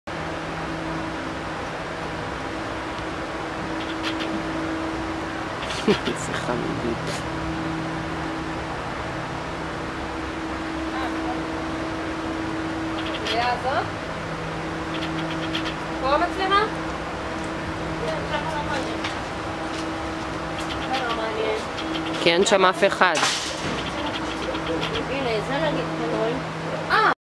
כדור שוקולד
חתלתולון שהוא פשוט כדור מתוק של שוקולד, כל היום רק מבקש עוד ועוד פינוקים, שלמות של גורון שיהפוך כל מאמצים למאושרים. שחורטיאניס בן 5 חודשים, הוא מחכה בבית המחסה שלנו בכניסה לכפר רות 3703* שלוחה 3